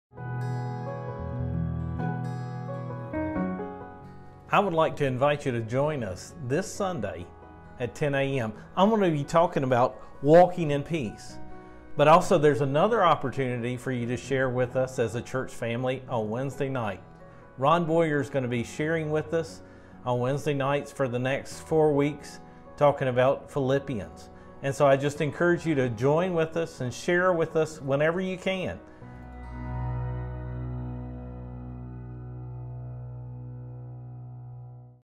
Promo Online Services
Join us on Sunday at 10am for our online service with Pastor Tim Wright. He will be talking about "Walking in Peace". Also, tune in Wednesday at 7pm. A 4-week study on Philippians begins with Ron Borer.